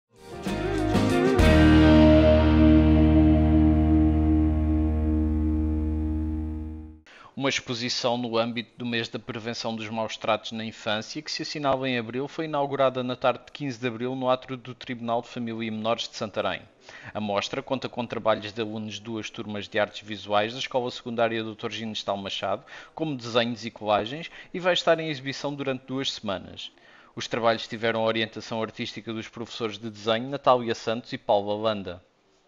Exposição no Tribunal de Família e Menores em Santarém sobre maus-tratos na infância
O átrio do Tribunal de Família e Menores de Santarém tem em exibição durante quinze dias a exposição sobre os maus-tratos na infância com trabalhos de alunos da Escola Secundária Dr. Ginestal Machado.

Uma exposição no âmbito do Mês da Prevenção dos Maus-Tratos na Infância, que se assinala em Abril, foi inaugurada na tarde de 15 de Abril no átrio do Tribunal de Família e Menores de Santarém. A mostra conta com trabalhos de alunos de duas turmas de artes visuais da Escola Secundária Dr. Ginestal Machado, como desenhos e colagens, e vai estar em exibição durante duas semanas. Os trabalhos tiveram orientação artística dos professores de desenho Natália Santos e Paulo Lalanda.